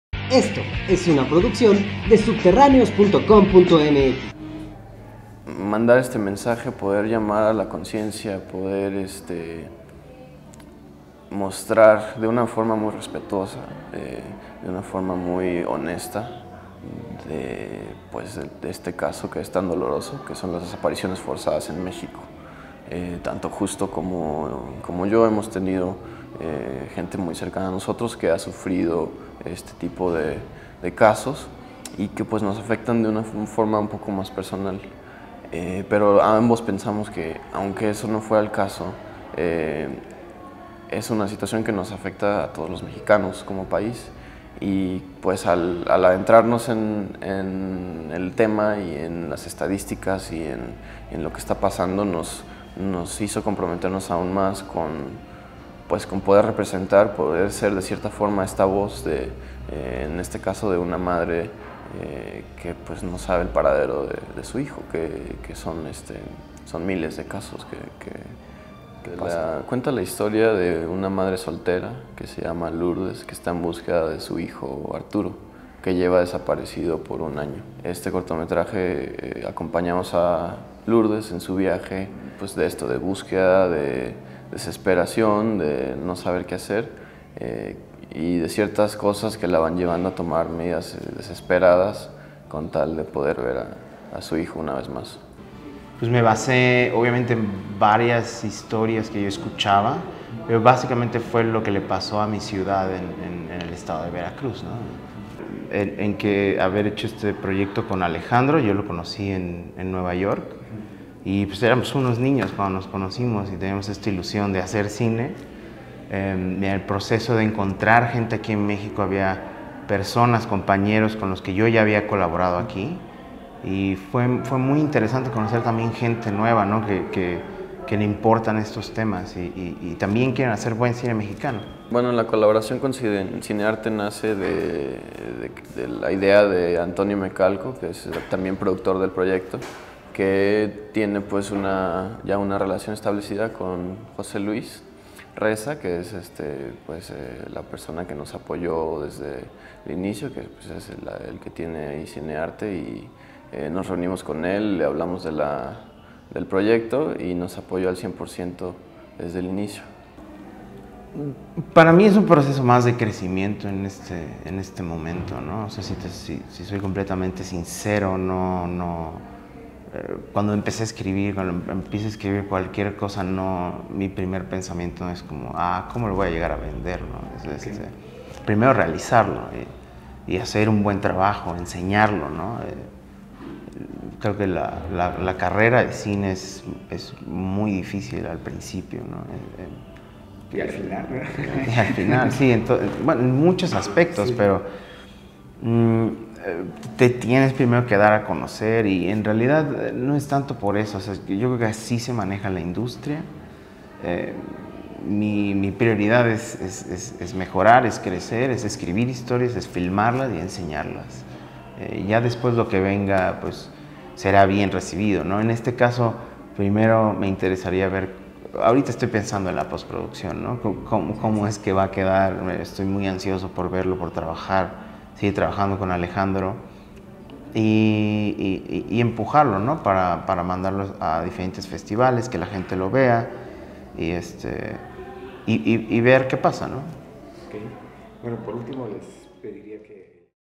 Se filma en Puebla, Desaparecidos
Un cortometraje que expone el dolor de una madre

José Corte/Hugo Cabrera/Subterráneos

*En 2016 se registraron más de 29 mil desaparecidos en México

Puebla, Puebla; 4 de abril 2917. "Se lo que significa que desaparezca un ser querido, por ello pensamos en ser activos en los problemas sociales. Desaparecidos cuenta la historia de Lourdes que busca a su hijo ausente. Es un enfoque familiar; es un tributo a las personas que no pueden volver a ver a sus hijos”, dice Alejandro Cortés, realizador cinematográfico que filmó en Puebla el cortometraje, como alumno de la School of Visual Arts, con especialización en cinematografía, en la ciudad de Nueva York.

El cortometraje grabado también en Atlixco, Puebla, tuvo el apoyo de Cinearte, la escuela de cine que dirige José Luis Reza.

Descarga nuestra App y recibe información de cultura rock y alternativas en tu dispositivo móvil. Busca 'Subterráneos' en Google Play o Apple Store